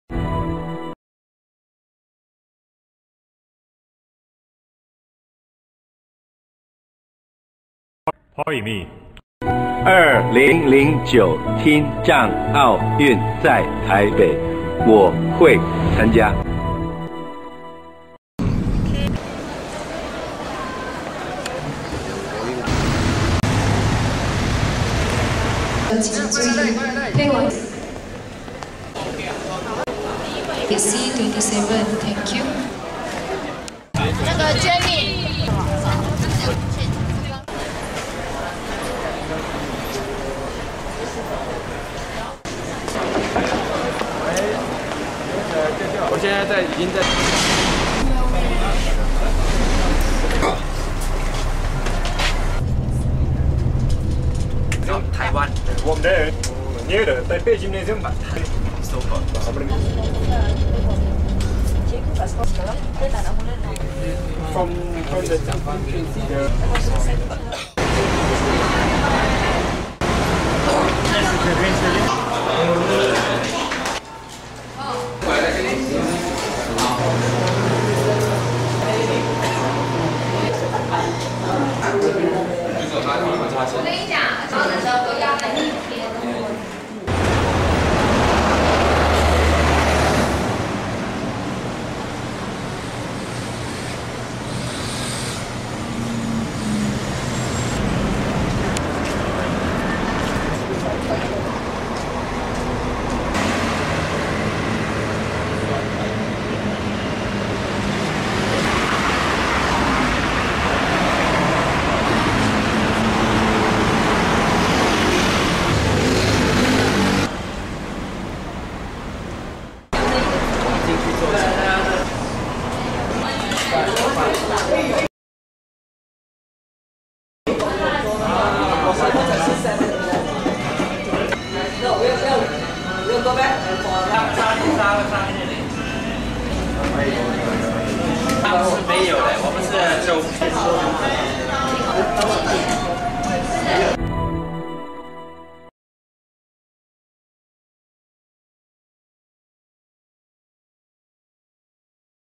Malaysian Team to Deaflympic 2009
A Journal Video of Malaysian Team to Deaflympic in Taipie 2009, 21th Summer Deaflympic 2009, and the Offical website of MSFD